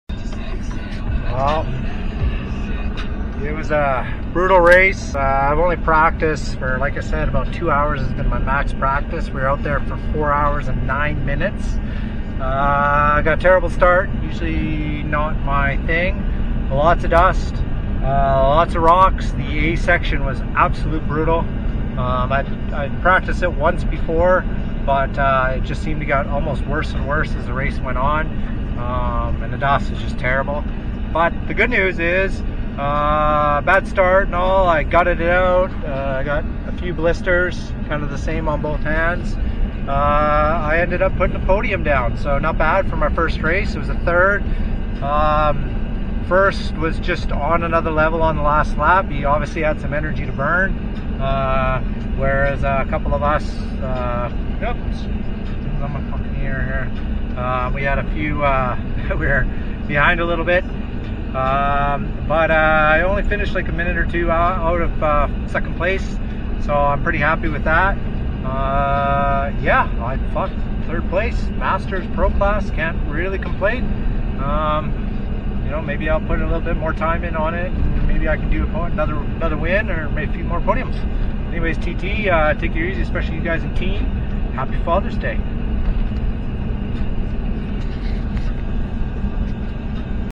Post Race Result
Brutal and dusty race. A little luck and I managed a podium finish after 4+hrs of racing.